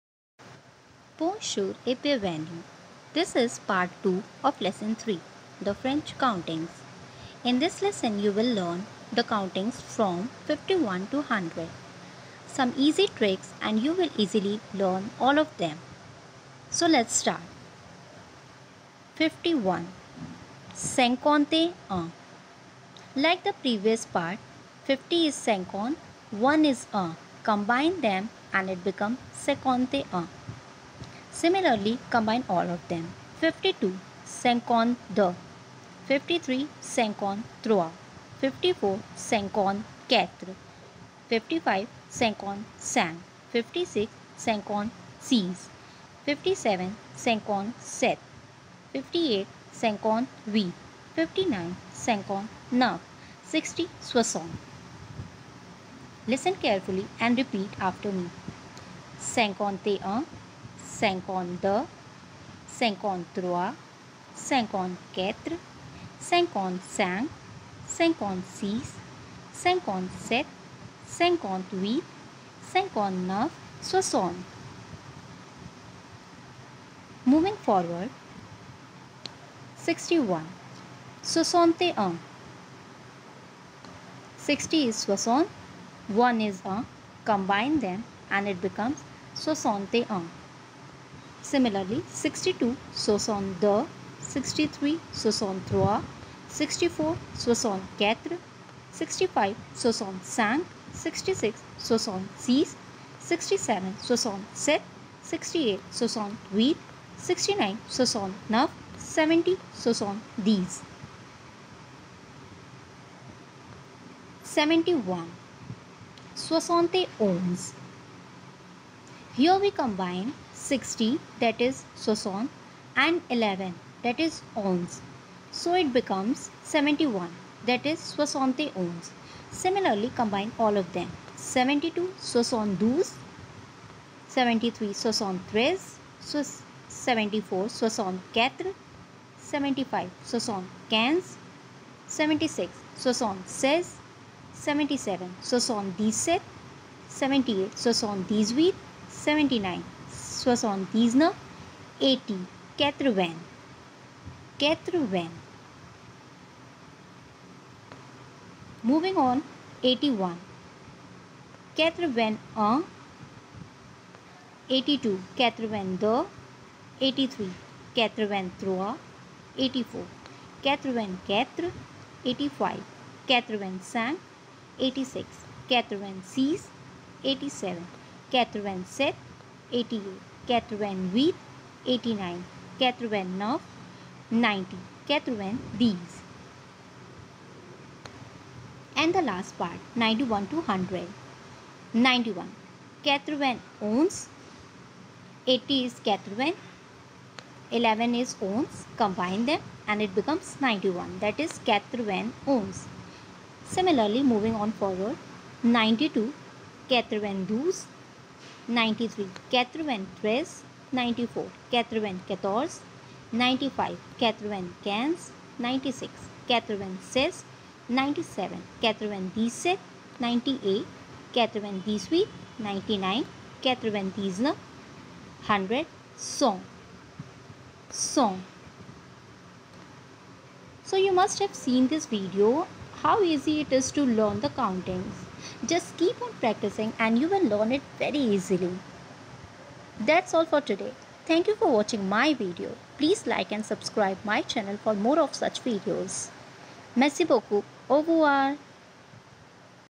French Countings Part 2 (51-100)| Countings in French language||Cinquante et un to Cent
Learn French Countings with simple tricks.